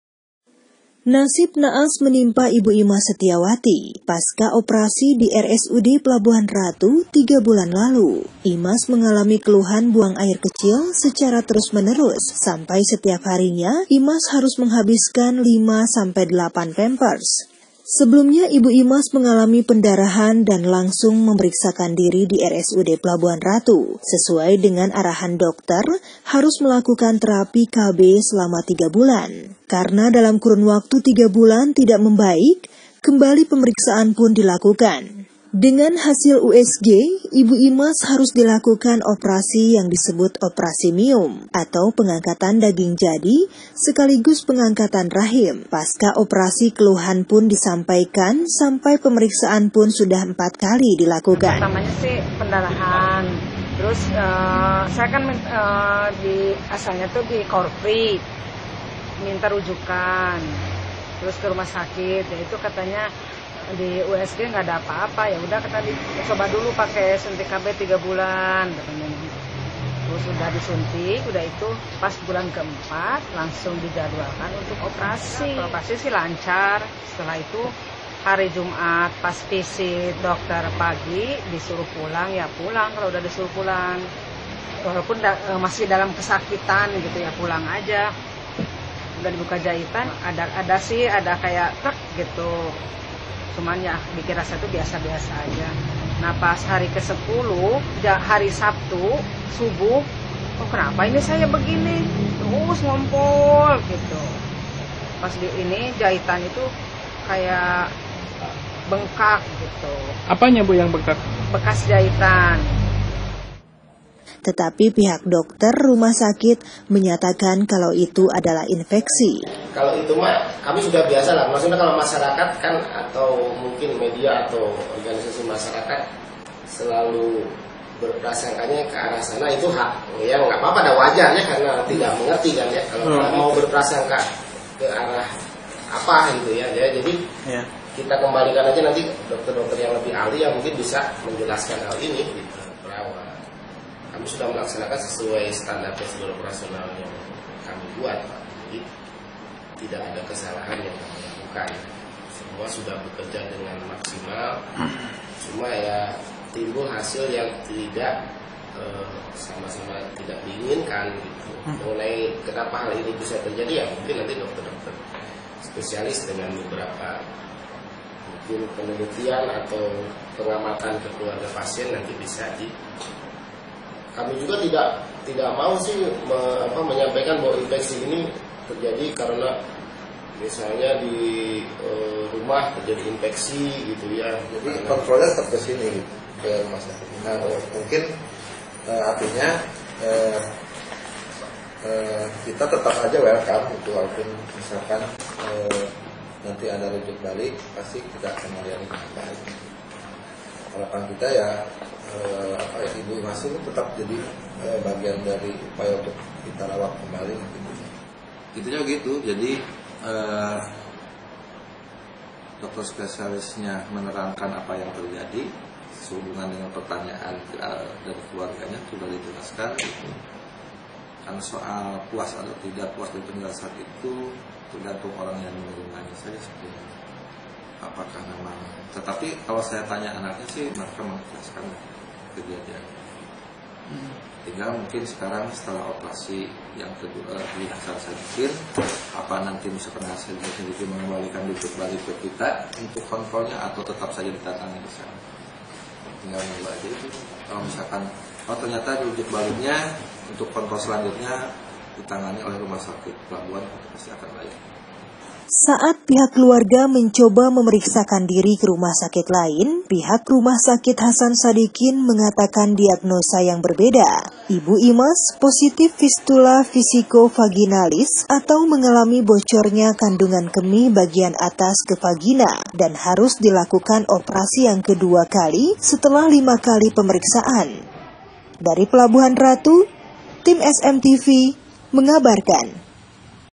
SEPUTAR SUKABUMI - PASIEN RSUD PLBHN RATU. KELUHKAN BUANG AIR KECIL TERUS MENERUS PASCA OPRASI
Liputan crew smtv